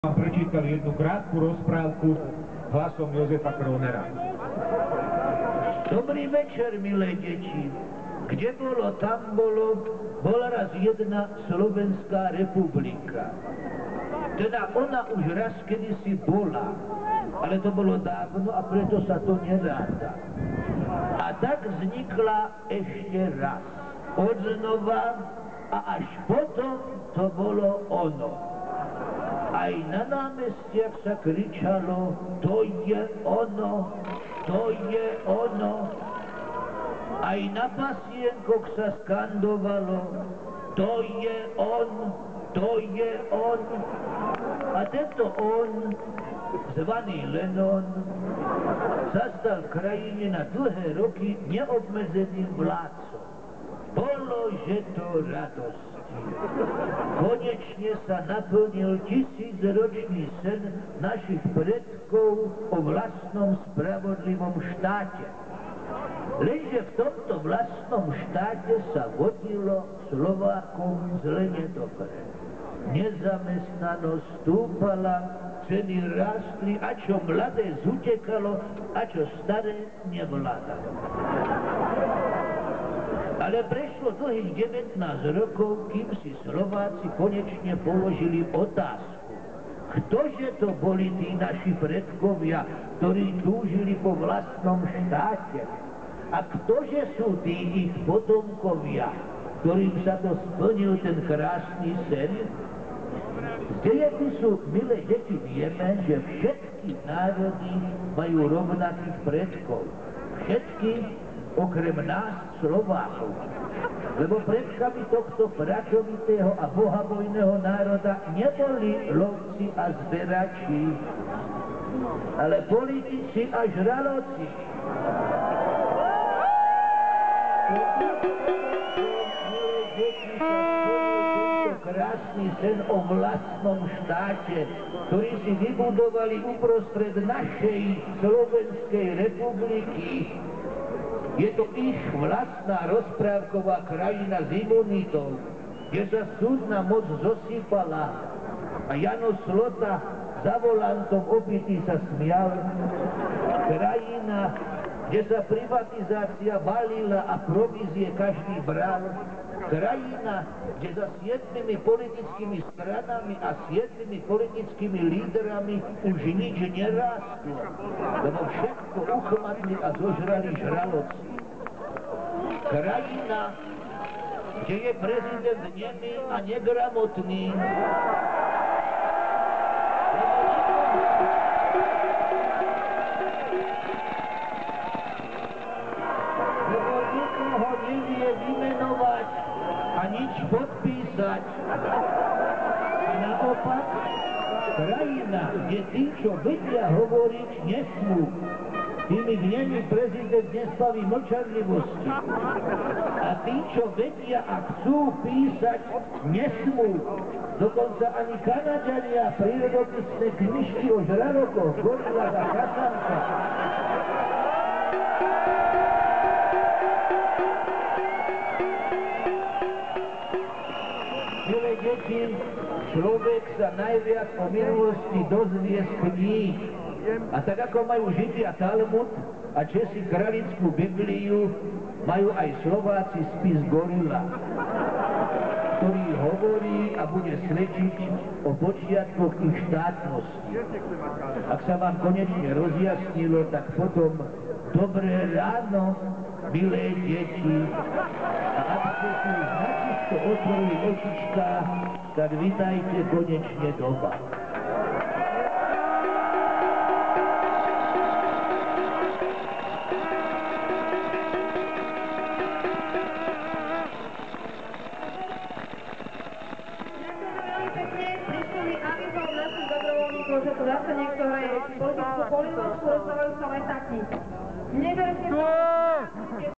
Protest Gorila 3, Rasťo Piško a rozprávka na dobré ráno
Rasto Pisko hlasom Jozefa Kronera rozprava rozpravku na dobre rano.
(Bratislava, Namestie SNP, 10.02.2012 o priblizne 17h 09m)